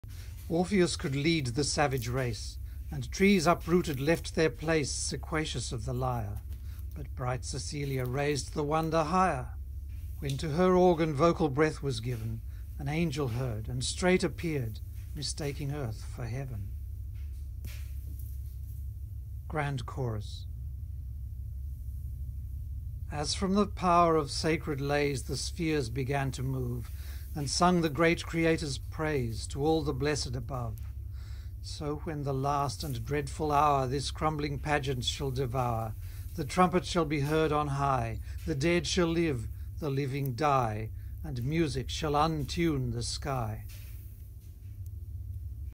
Pop up Apocalypse
A pop-up vision of Dublin's Arran Quay in a post-collapse world, part of a larger architectural project exploring the nature of human settlement when civil society as we know it has ceased to exist.

The audio is a part of a recital of the poem, 'A Song for St Cecilia's Day', by John Dryden, composed in 1687. Attribution: a poemscafe.com production